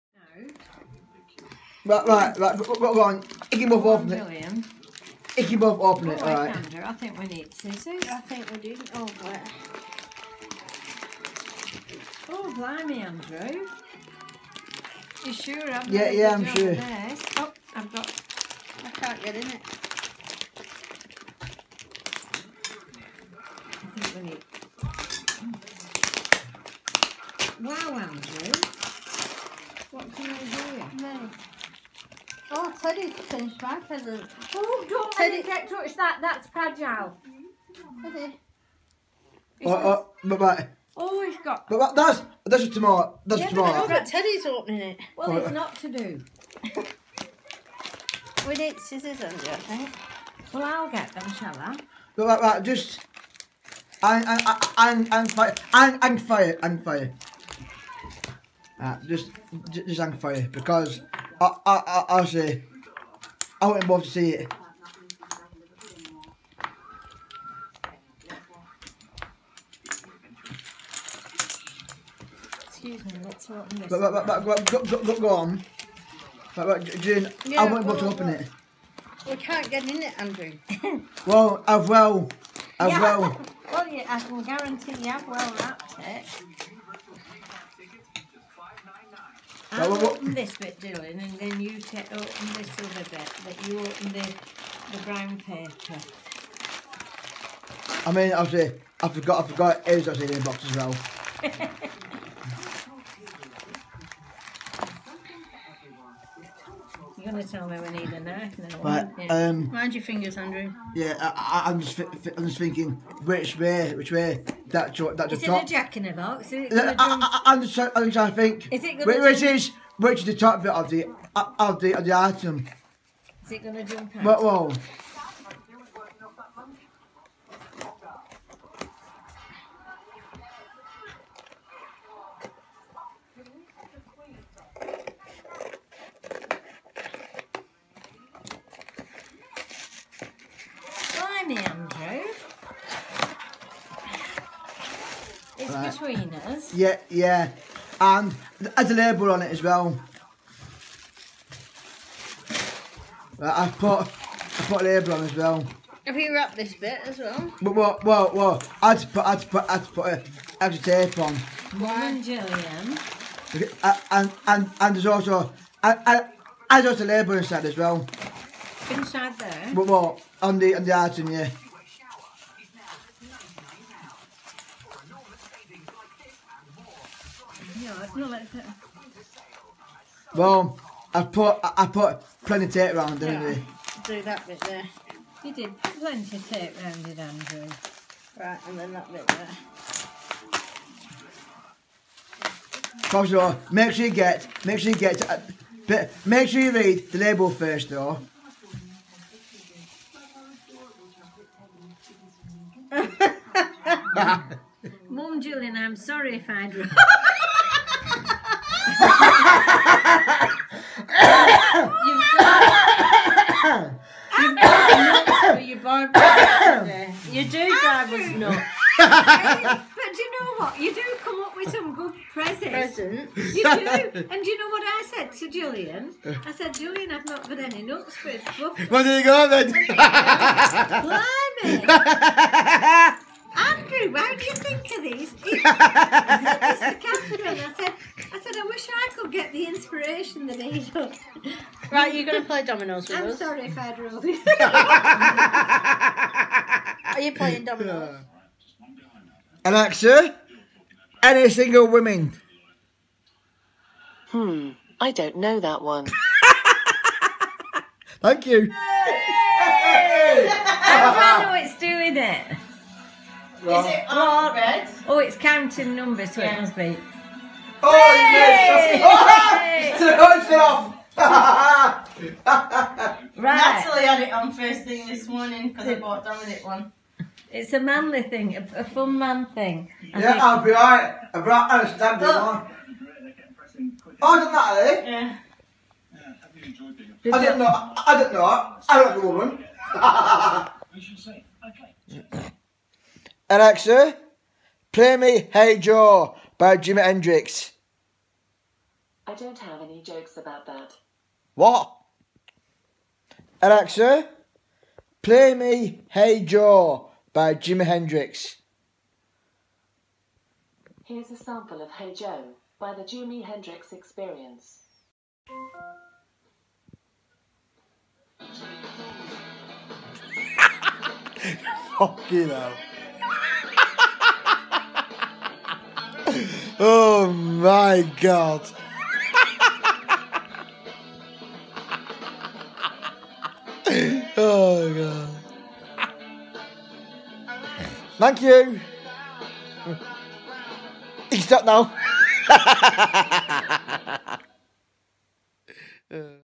Christmas Eve / Christmas Day 24th 25th December 2017
Filmed on iPhone 6 mine and my mums today has been absolutely funny Christmas 🎄 ha today say I was on diazepam to keep me chilled etc !
Camera vids to come soon
Enjoy for now and hope for some more subscribers before new year pls
And of course like